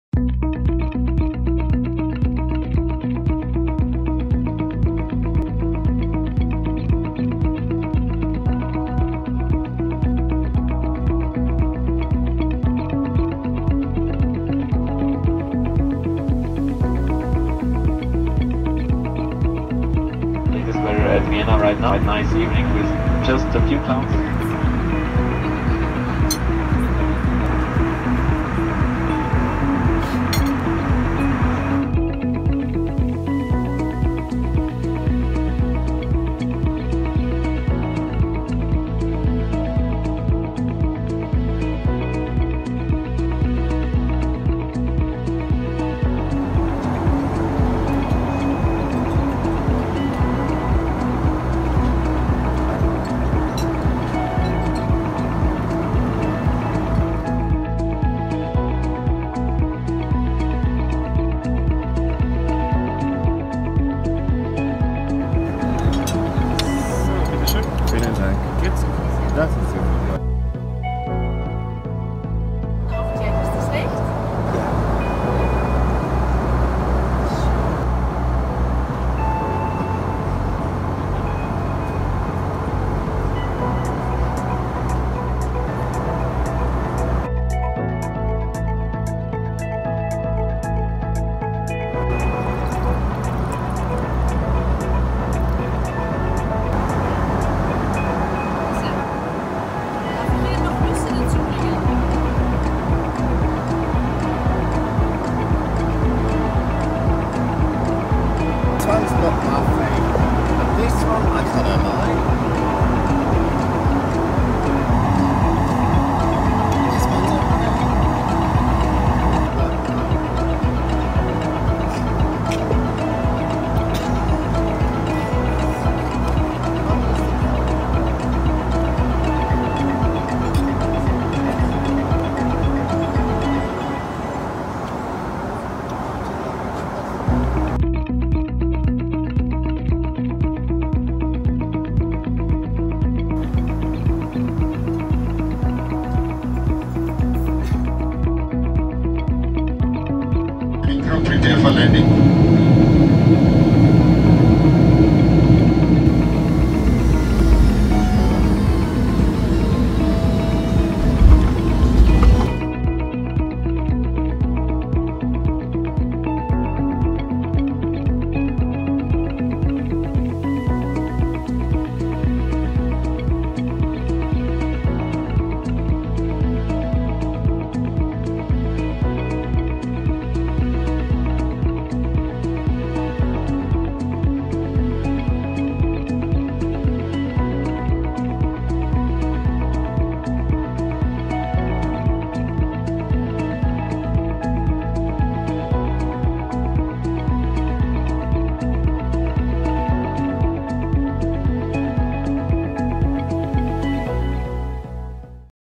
FOOD & FUN on Austrian Airlines Business Class, Berlin - Vienna - Bangkok
This is not a typical flight review of Austrian Airlines Business Class from Berlin - Vienna - Bangkok, but concentrates solely on FOOD & FUN. One particular highlight of this review is the in-flight wine tasting!

For detailed information and reviews, please view other material.